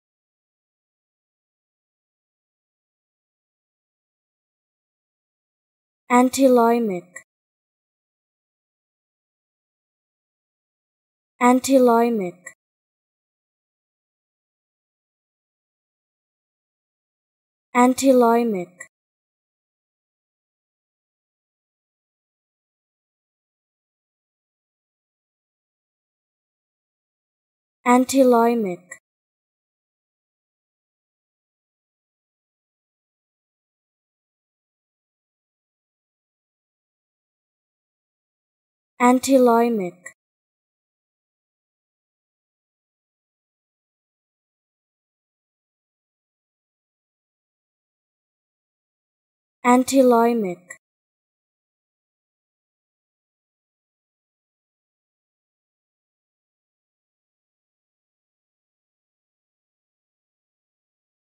How To Say Antiloimic
Learn how to say Antiloimic with EmmaSaying free pronunciation tutorials.